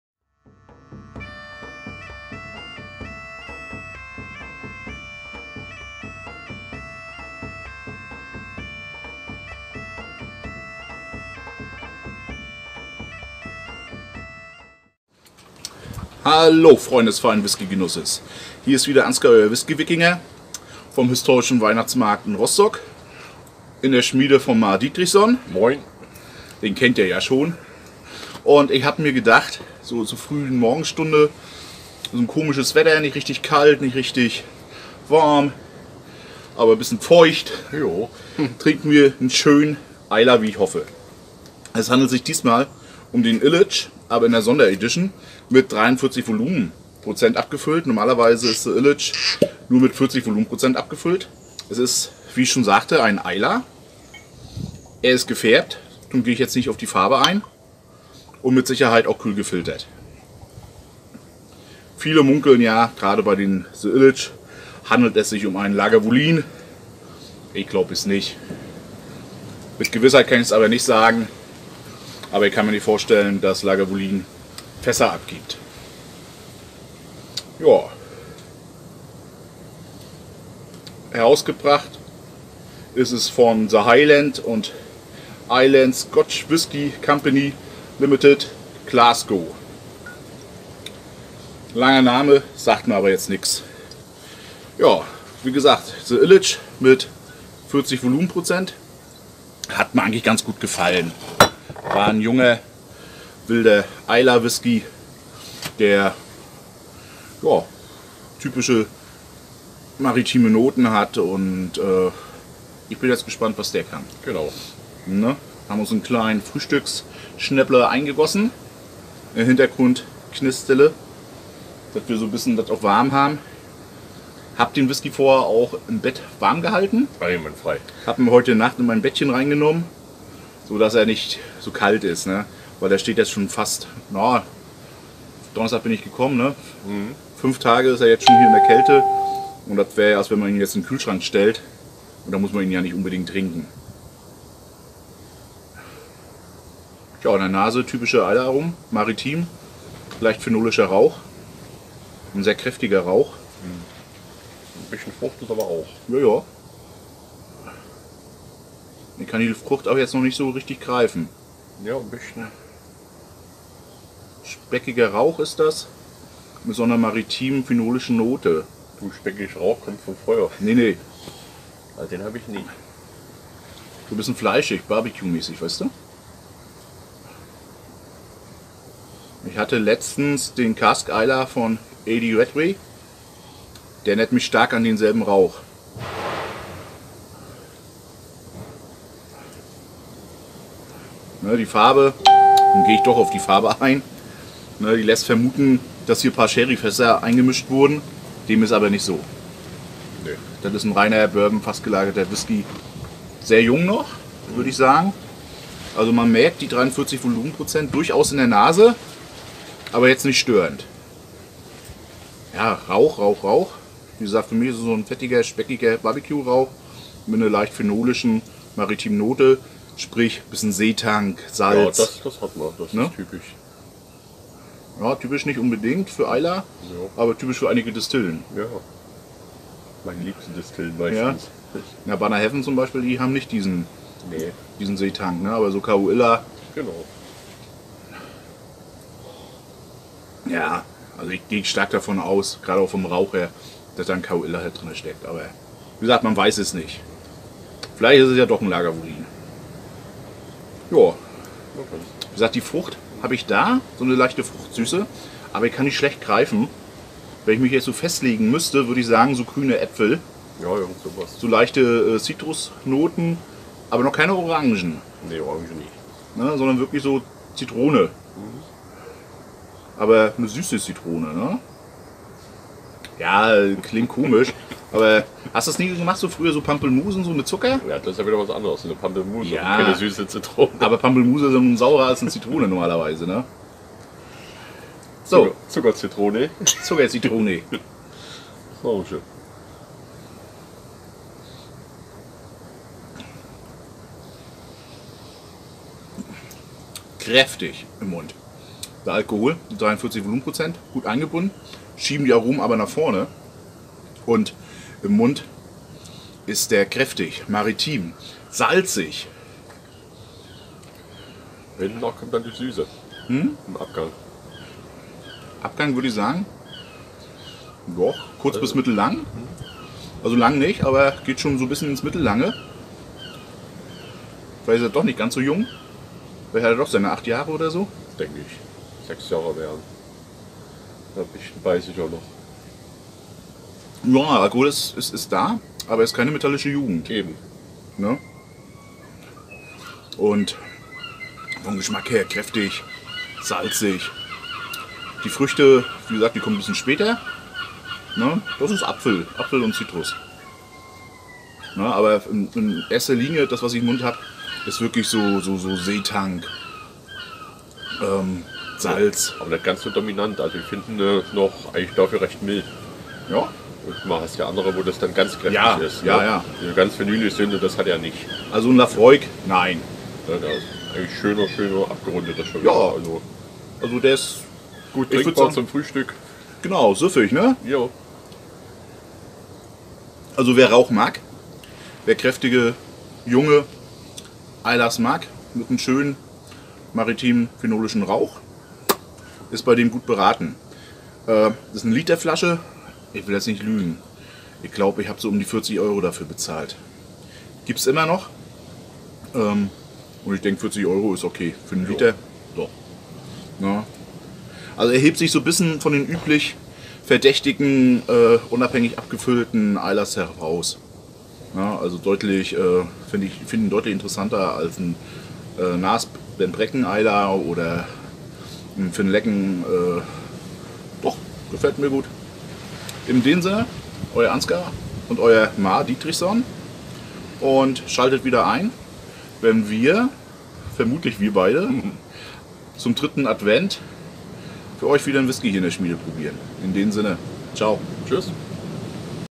Ileach 43% # Whiskyverkostung 234
Ahoi, heute verkoste ich auf dem historischen Weihnachtsmarkt in Rostock einen unabhängig abgefüllten Islay ... die Brennerei ist nicht bekannt!